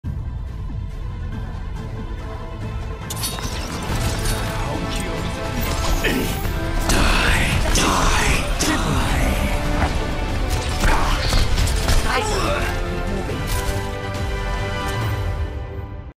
Reaper is best ninja. ...Believe it.
Edgelord shows these ninja wannabes exactly what he thinks of them.